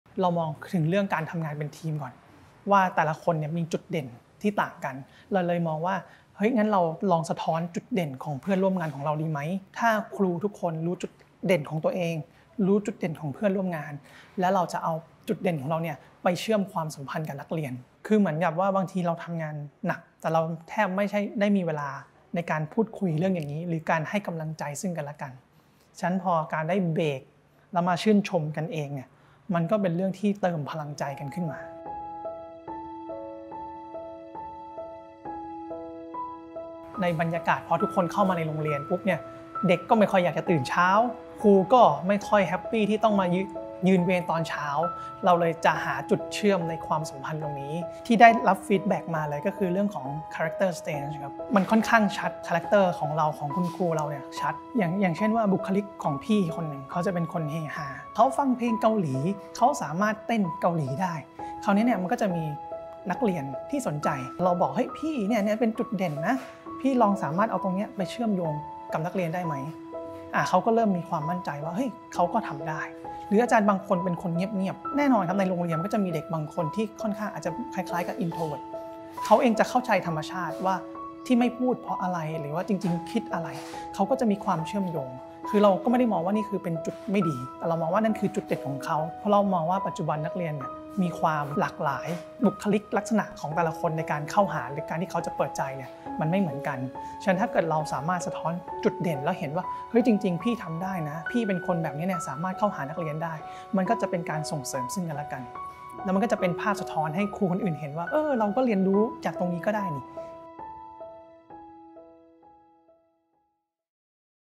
ค้นหาจุดเด่นของกันและกัน เชื่อมสัมพันธ์เชิงบวก - เปิดสวิตซ์ ติด Skill | Famskool
เปิดสวิตซ์ ติด Skill -  ถ้าครูรู้จุดเด่นของตัวเอง รู้จุดเด่นของเพื่อนร่วมงาน เราก็จะนำจุดเด่นไปเชื่อมสัมพันธ์กับนักเรียนได้  กรัณย์ รุ่งทวีสิน

ชวนพูดคุยหลังกิจกรรม 'เสริมสร้างจิตวิทยาเชิงบวก'  ที่ทาง Famskool พยายามสร้างทั้งเครื่องมือ สร้างทั้งกระบวนการฝึกอบรมที่เอื้อไปให้คุณครูได้ฝึกเรื่องเหล่านี้ เพื่อนำไปสร้างสัมพันธภาพที่ดีในห้องเรียน
สนับสนุนโดย FamSkool Life Education Thailand และ สสส. (สำนักงานกองทุนสนับสนุนการสร้างเสริมสุขภาพ) : ThaiHealth